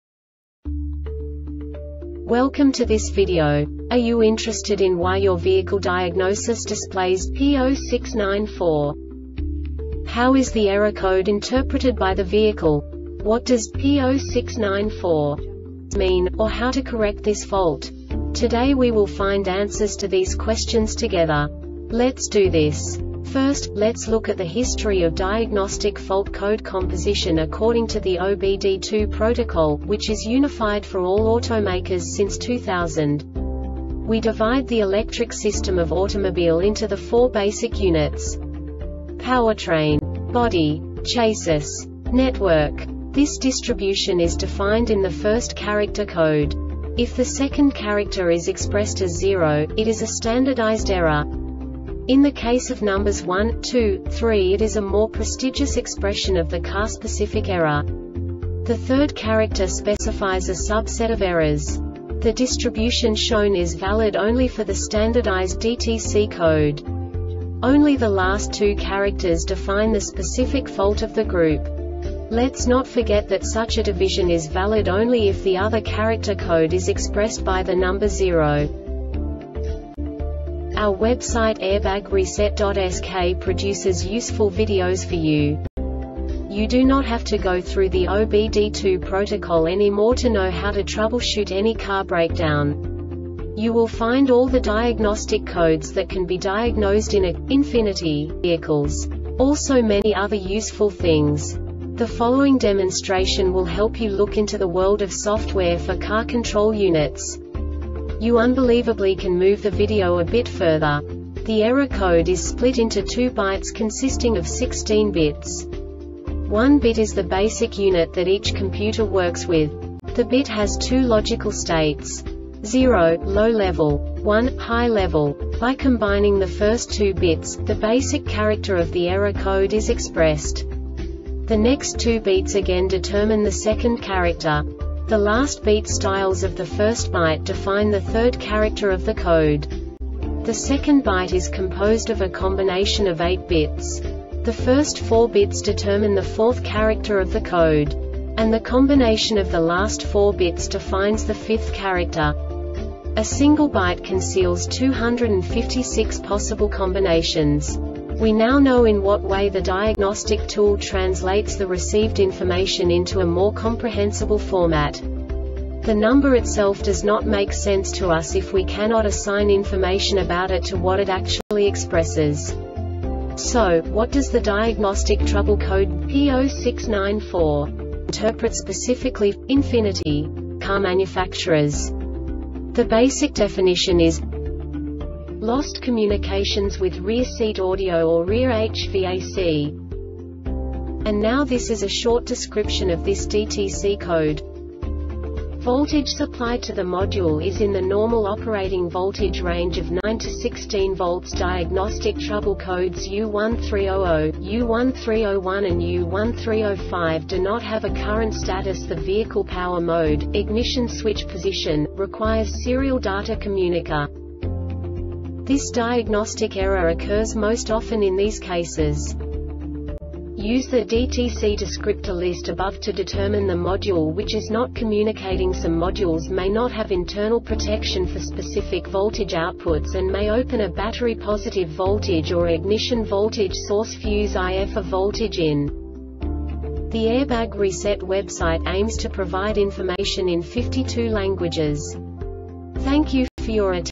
DTC Ford P0694 Short Explanation
Contents:
0:21 Basic DTC analysis according to OBD2 protocol standard.
1:48 Insight into programming
2:58 A diagnostically understandable description of the Diagnostic Trouble Code
3:05 Basic error definition

Make:
Ford
Code:
P0694
Definition:
Lost Communications With Rear Seat Audio or Rear HVAC
Description: